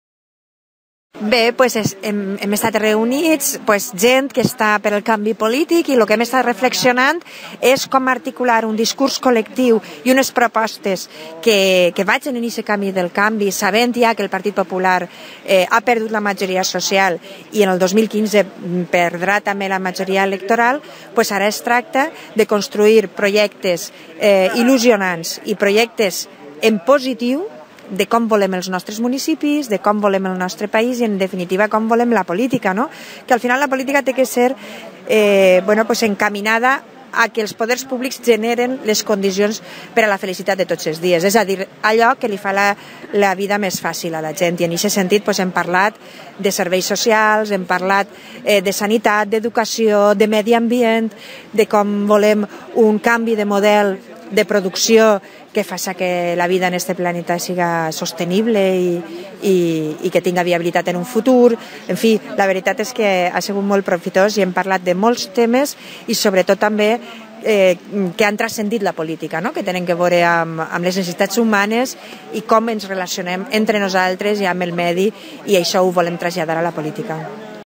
Oltra recull les preocupacions de simpatitzants de Compromís de Peníscola
La diputada de Compromís Mònica Oltra va reunir-se en la vesprada de dilluns amb simpatitzants i militants de Peníscola partidaris d’un canvi polític al País Valencià “reflexionant sobre com articular un discurs alternatiu, projectes il·lusionants i propostes en positiu que vagen en la línia del canvi sabent que el PP ha perdut ja la majoria social i que al 2015 perdrà la majoria electoral”, va declarar Oltra.